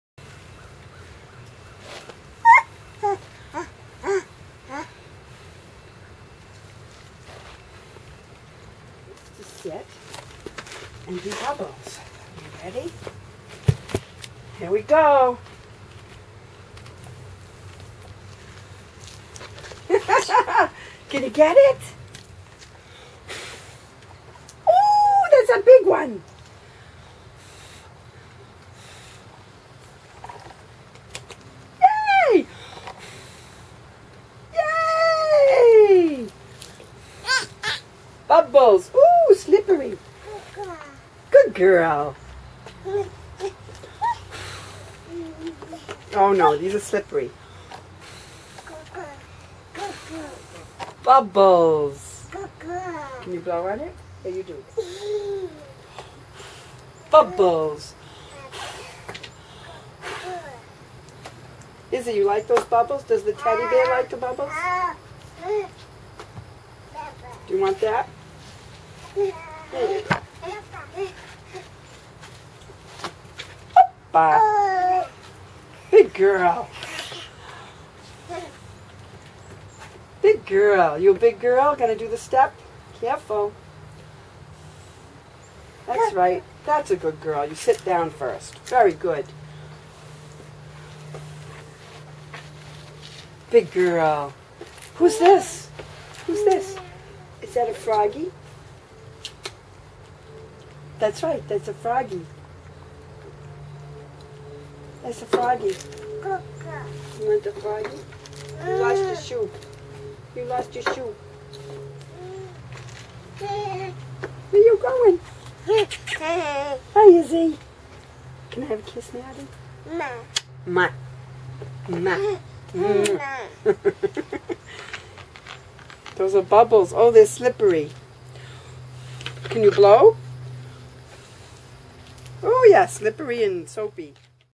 Blowing Bubbles!
Bubbles are twice the fun in our house!!  The girls are 15 months here.  They are so adorable!!